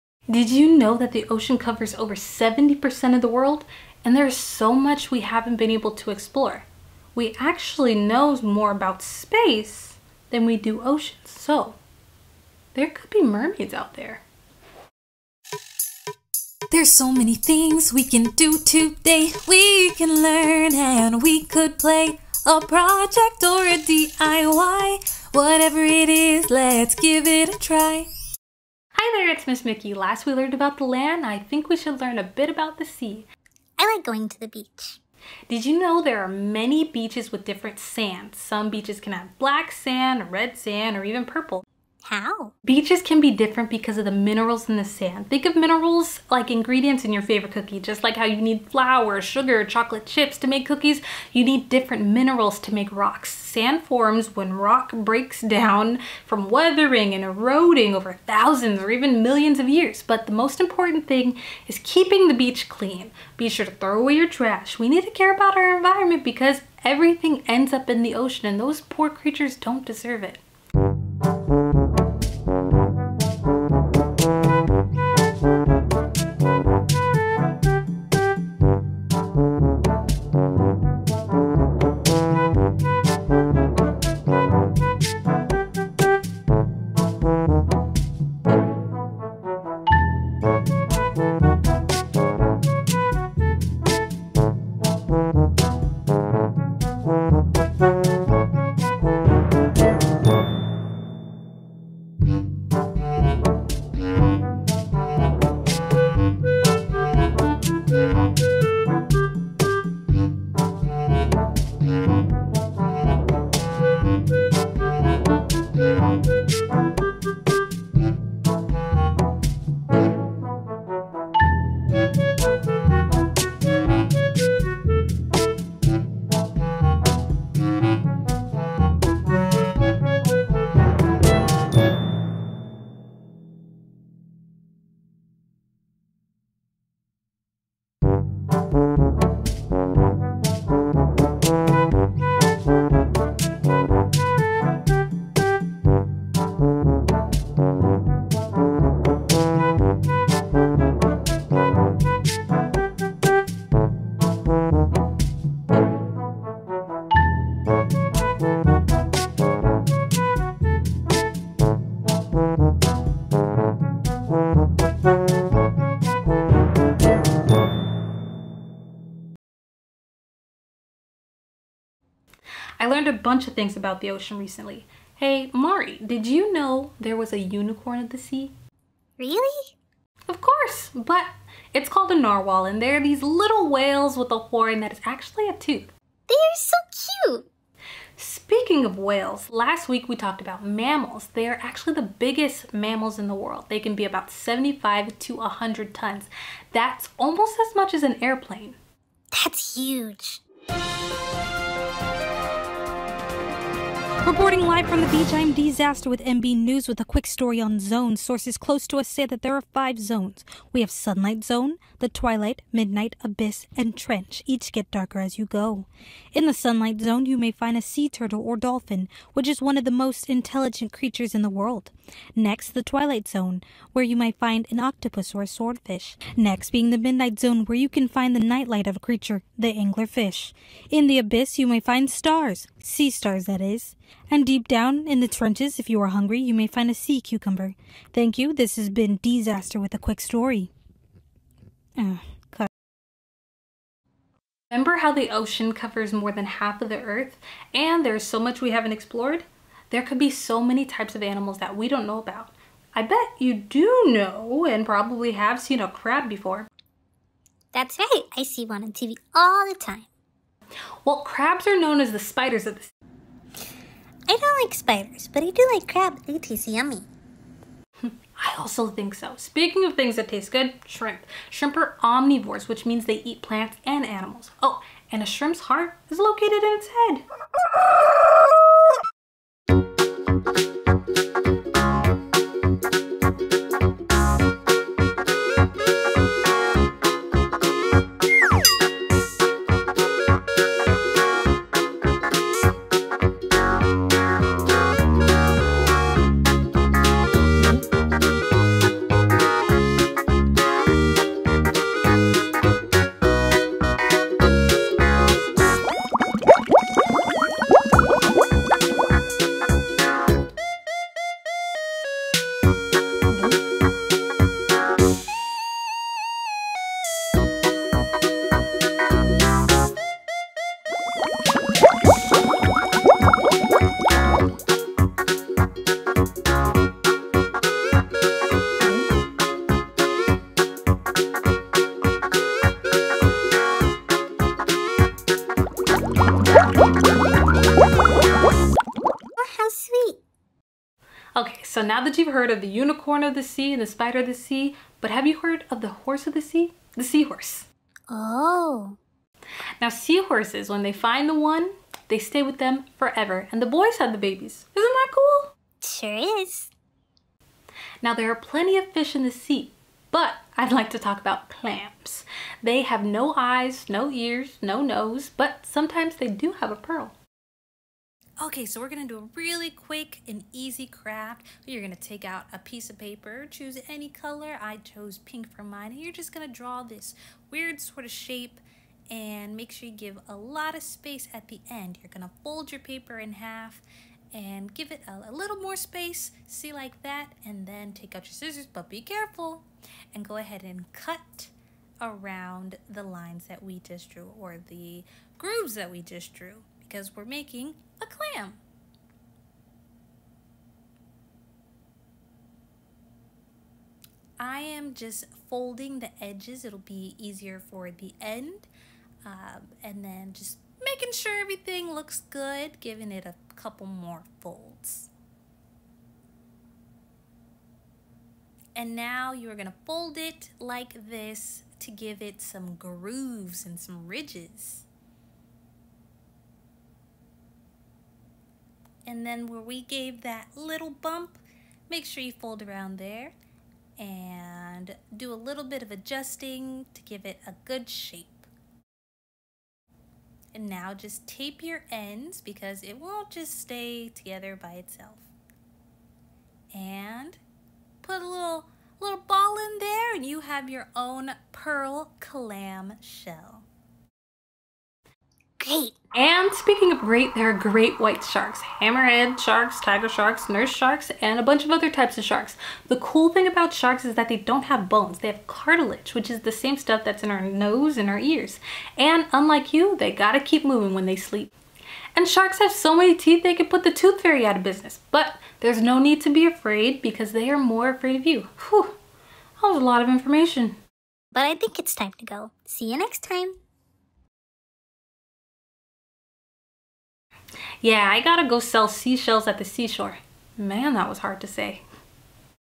Let’s learn about the Ocean
Dive Into the Ocean with Ms. Miki & Maree! | Sea Creatures, Fun Facts & Beach Adventures

Did you know the ocean covers more than 70% of the Earth—and we’ve only explored a tiny part of it? That means there could still be mysteries like mermaids hiding beneath the waves!

Join Ms. Miki and Maree in this exciting and educational adventure as we learn all about the ocean, sea creatures, and how to take care of our beaches! From sandy shores to the deepest trenches, we explore:

PLUS!A surprise report from Dee Zaster with MB News
A quick game of "Cereal Scramble"
 DIY ocean fun

This episode is packed with fun facts, ocean science, and important messages about protecting our environment. It’s perfect for kids, parents, and teachers looking for an entertaining way to learn about the amazing underwater world.